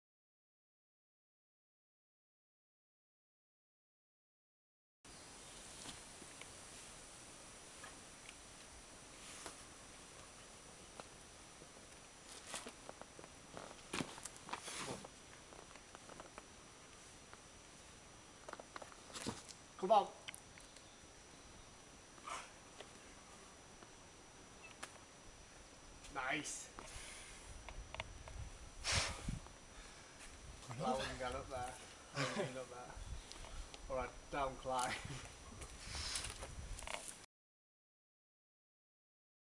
Kalashnikov (7b/c) Peninha, Sintra Portugal
Climb Newcastle owner Chris Graham on Kalashnikov (7b/c) at Peninha, Sintra.